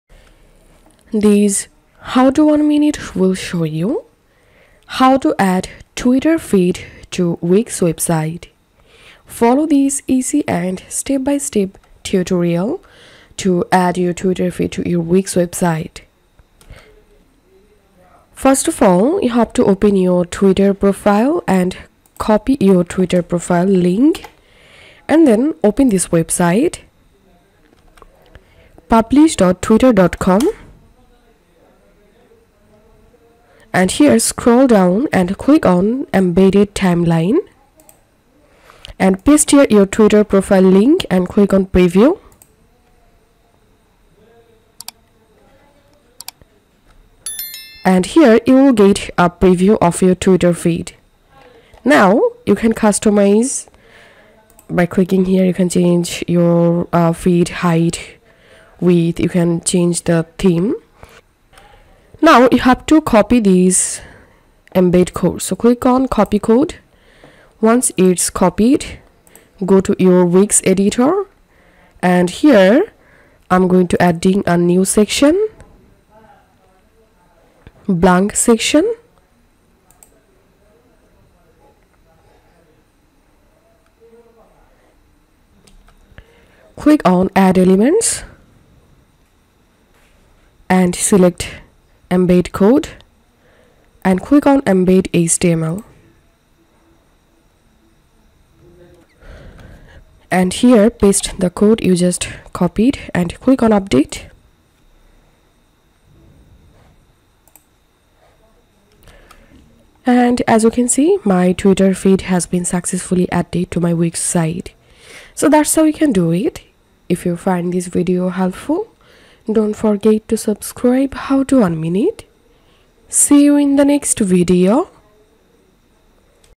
How to Add X (Twitter) Feed to Wix Website | Full Guide
Easily add X (Twitter) feed to your Wix website within minutes. Follow this detailed guide to embed your posts in multiple ways today.